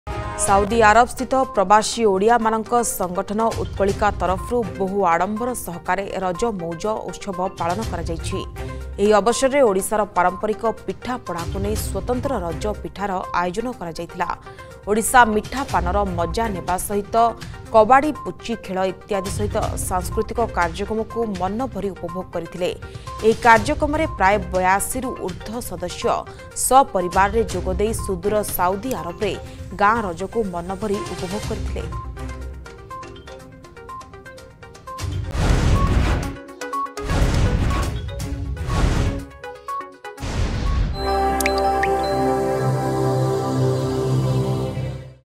Odia traditional festival Raja was celebrated in Saudi Arabia with a great joy
Utkalika, an organization of expatriate Odias in Saudi Arabia, has celebrated Raja festival with great pomp.